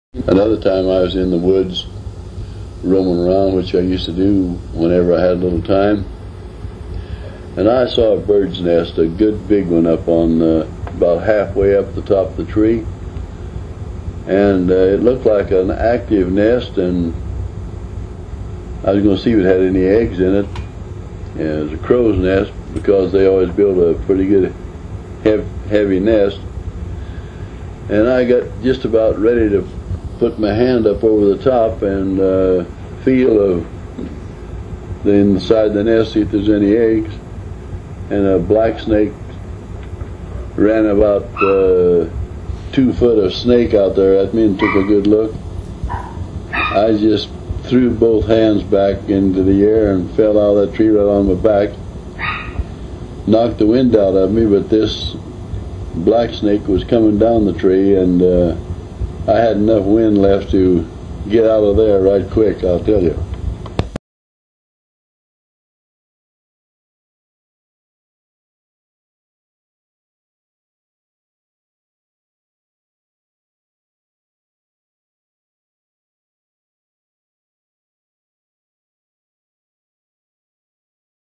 Grandpa Charlie Bangle and the Bird Nest
Missouri Farm boy Grandpa Bangle story about finding a bird's nest played with historic photos of Hickory County Missouri.  His book, "This is the Life! Stories of Grandpa Charlie Bangle," is available in Kindle and print at Amazon and in print